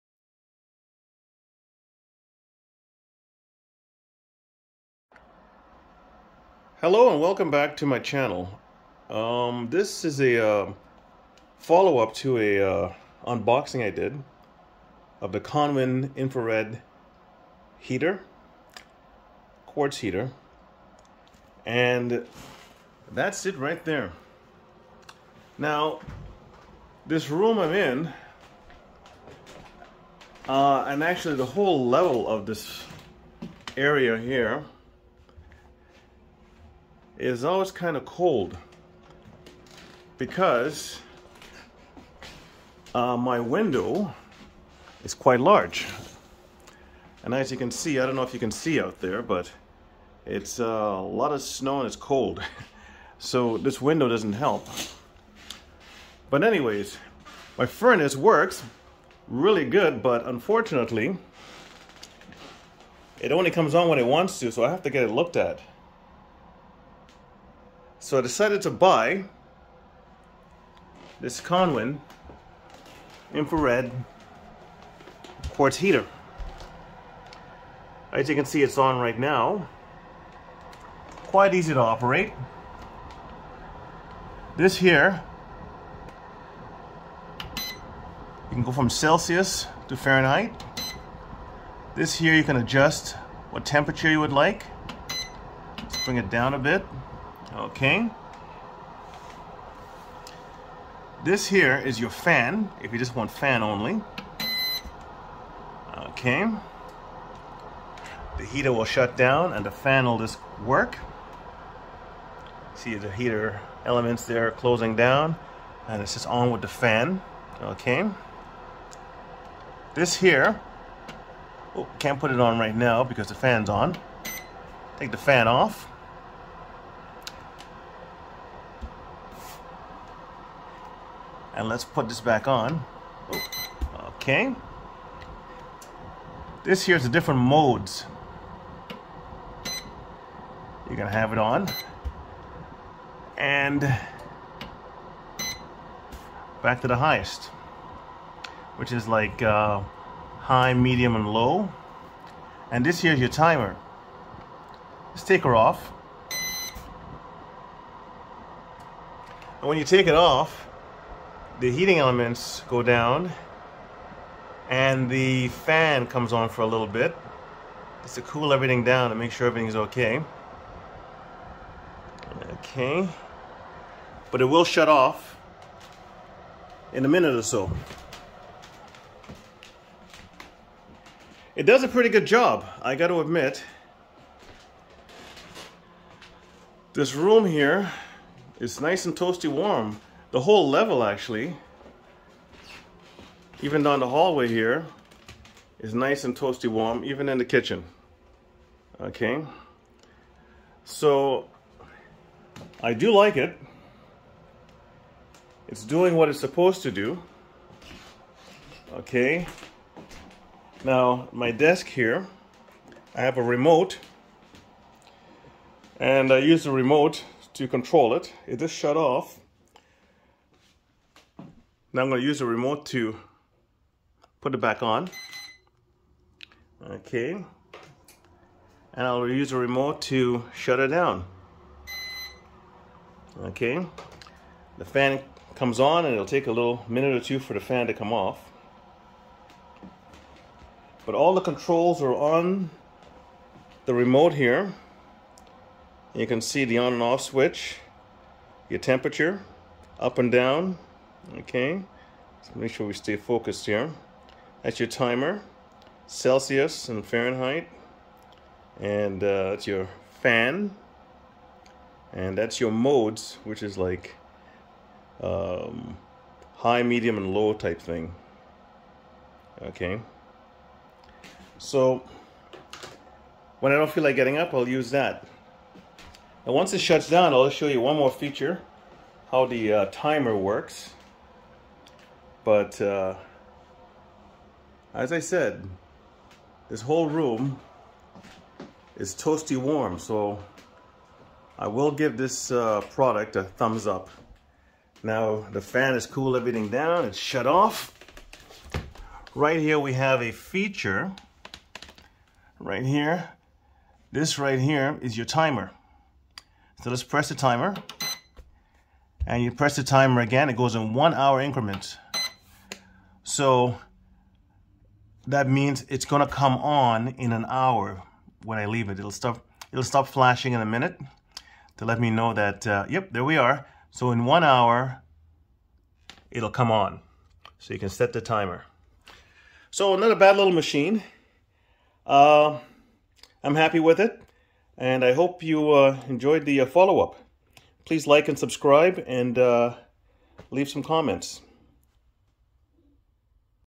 Konwin Infrared Zone Heating System Follow-Up Review
This is a follow-up to a unboxing I did of my Konwin heater

Dave How To 🤔